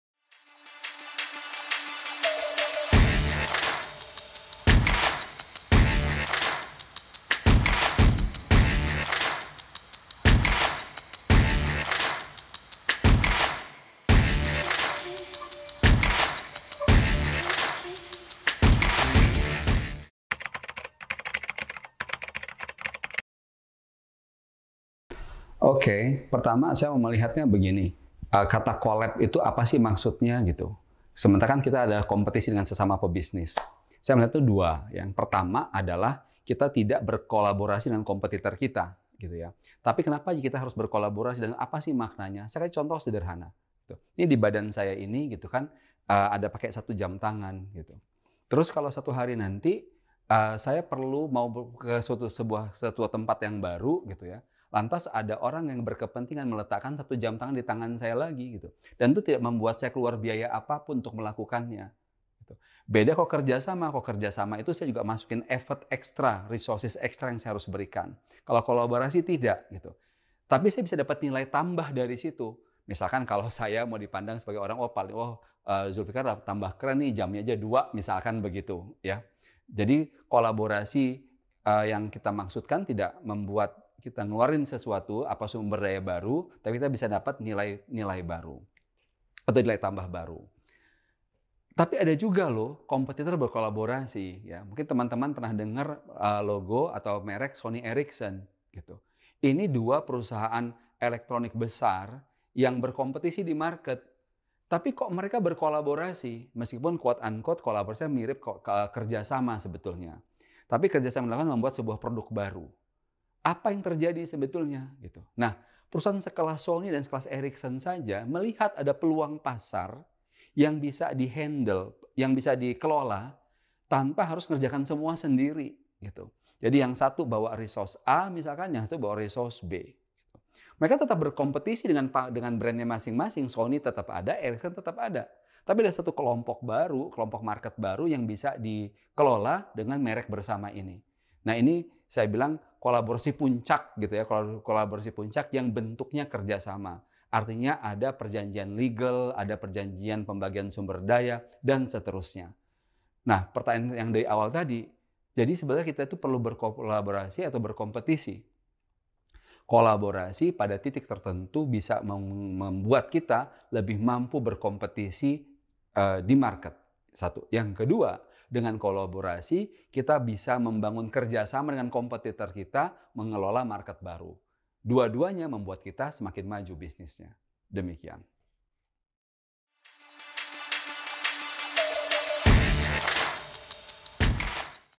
Ini Yang Penting Dari Kolaborasi | Seberapa Sulit Masalahmu S1 - E11
Seharusnya Brand akan saling Berkompetisi demi meraih tujuan masing - masing pihak. Tapi tidak menutup kemungkinan juga kalau tujuan perusahaan atau brand untuk mencapainya melalui kolaborasi yang disusun sangat ciamik. Mari kita lihat video berikut agar mencerahkan hari Anda.

Admin The New You Institute: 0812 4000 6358

Download The New You application